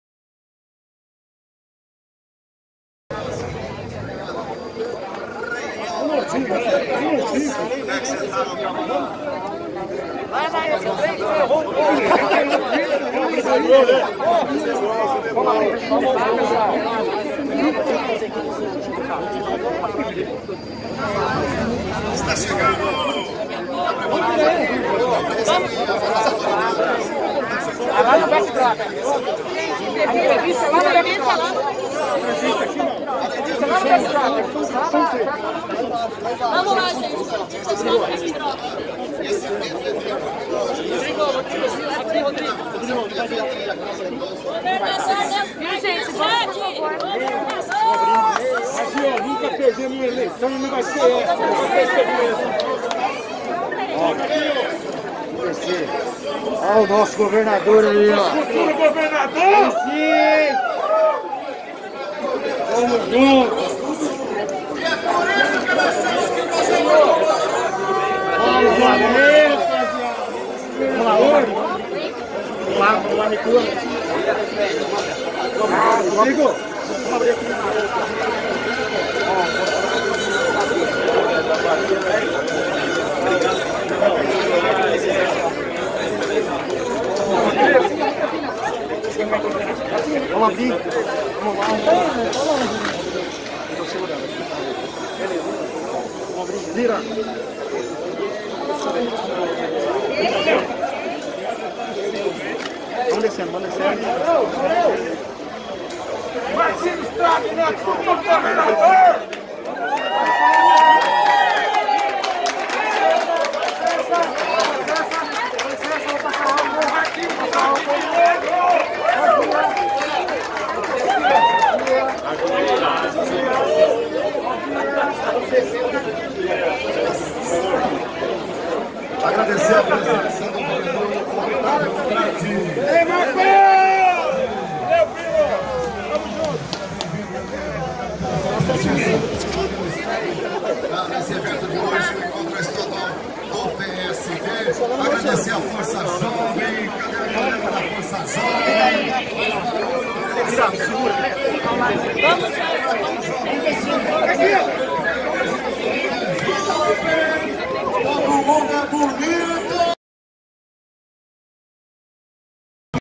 Lançamento da pré-candidatura ao governo do Estado do Prefeito de Campo Grande Marquinhos Trad
O evento de lançamento da pré-candidatura ao governo do Estado de MS aconteceu na manhã deste sábado (5) no Clube de Campo Associação Nipo Brasileira em Campo Grande.
Cobertura Tereré News.